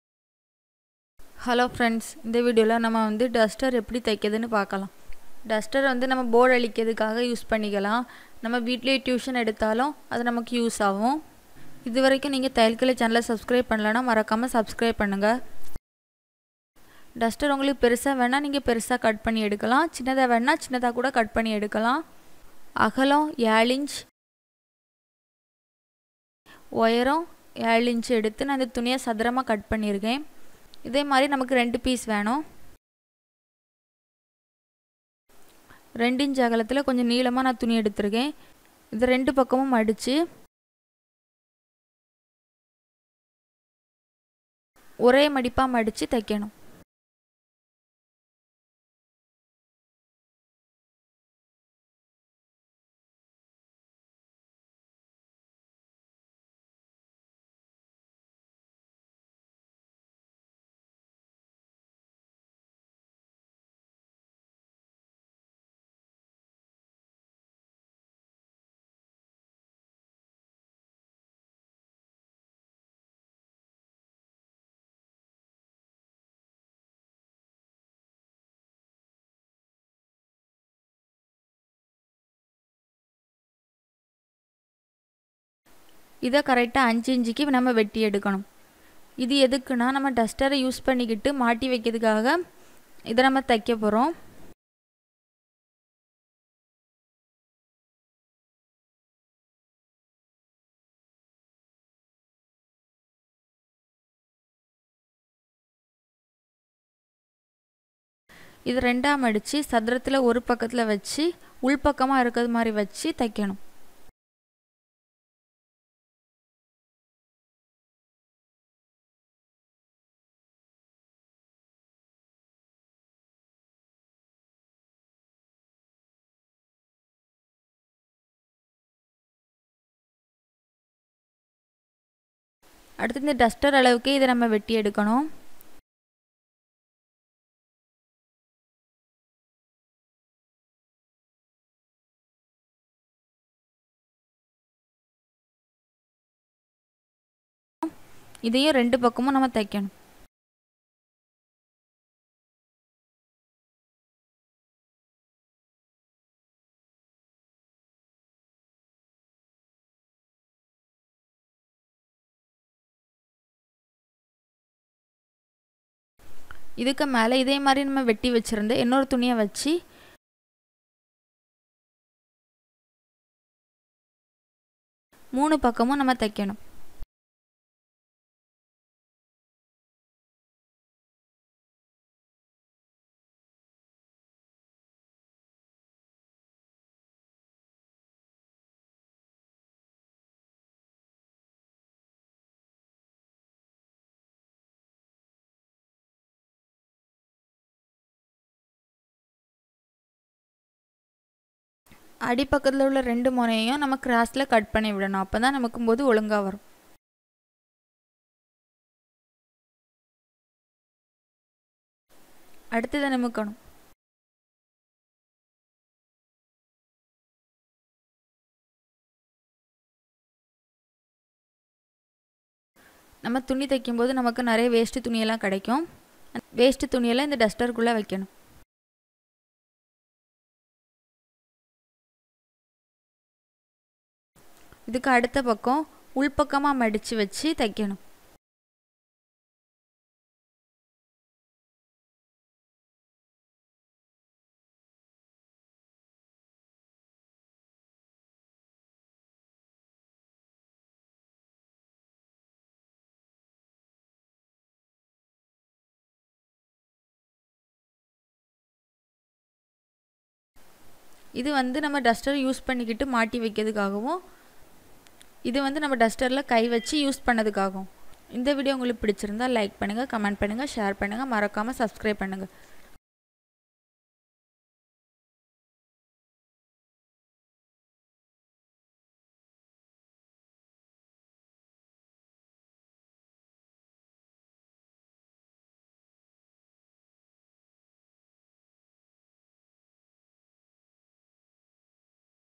How to Make (Chalkboard Eraser) Blackboard Duster - DIY
This Video is about How to Make (Chalkboard Eraser) Blackboard Duster in Tamil. You can make your Own Duster for School, College or Work. Earn a Good Respect by Contributing things like this to Your School. I've shown the Instruction about making Blackboard Duster. Watch This Full Video to Know More about it.